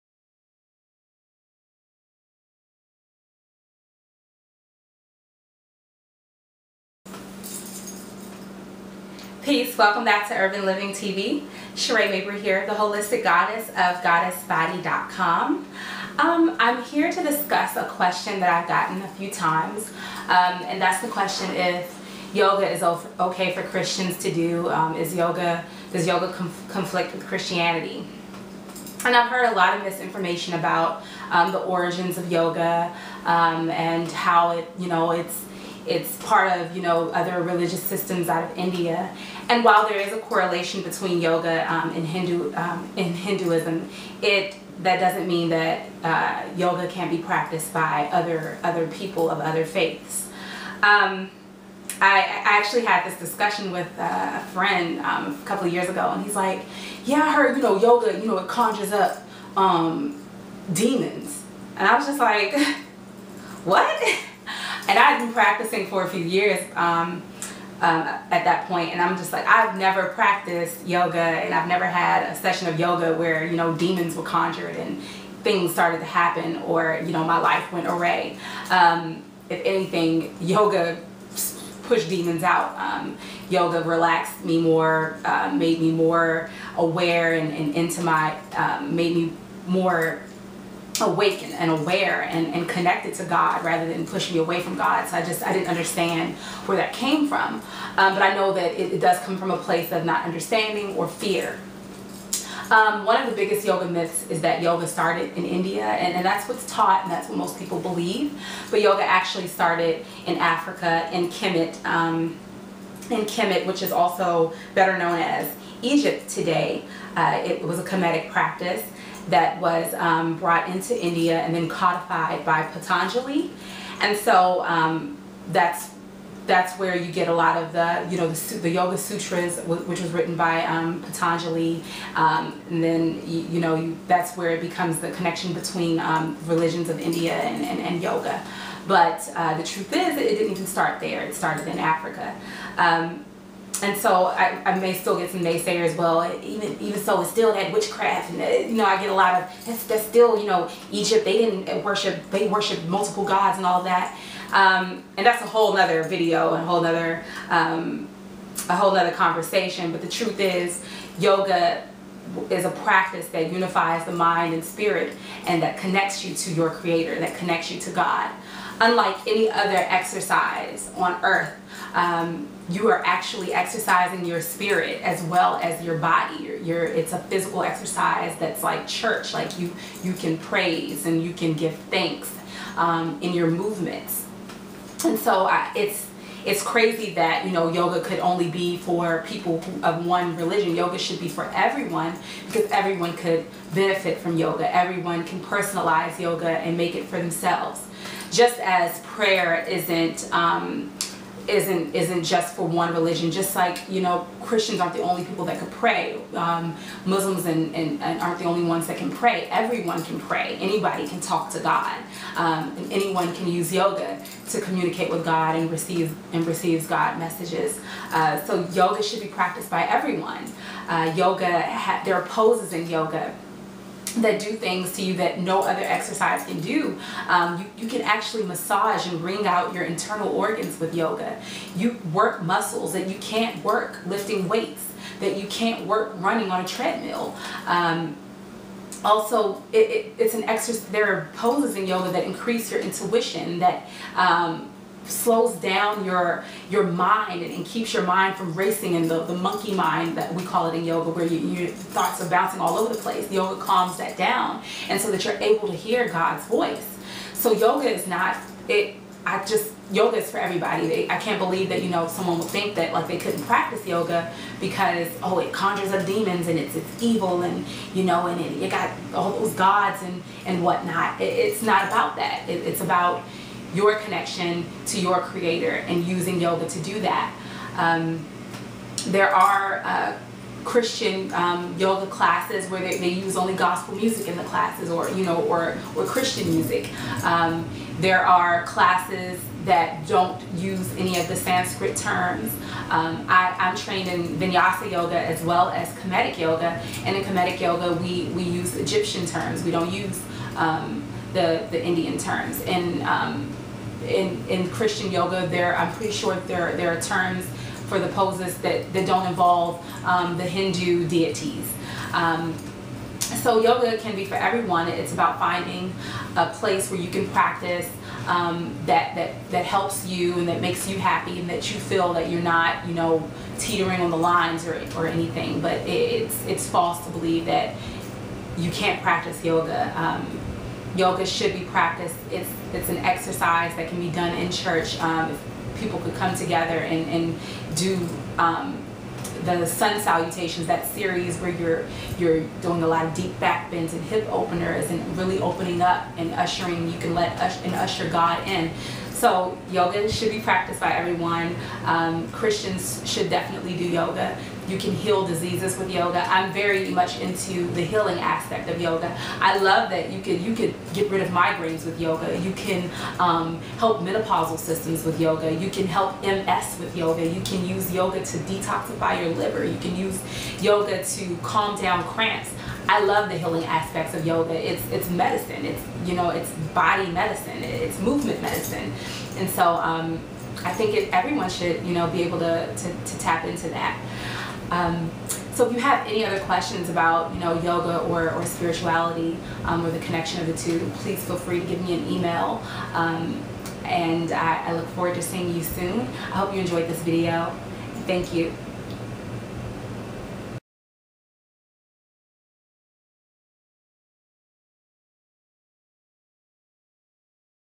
Can Christians Practice Yoga?
I answer the question, is it ok for Christians to practice yoga.